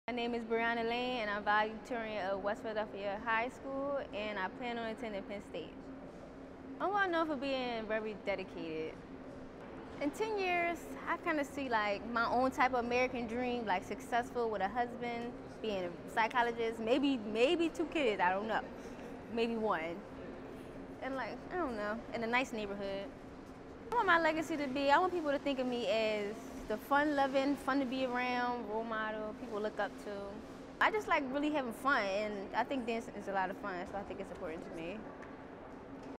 BRIANA LANE
School: West Philadelphia High School
Future Aspirations: Becoming a Psychologist
Interests: Cheerleading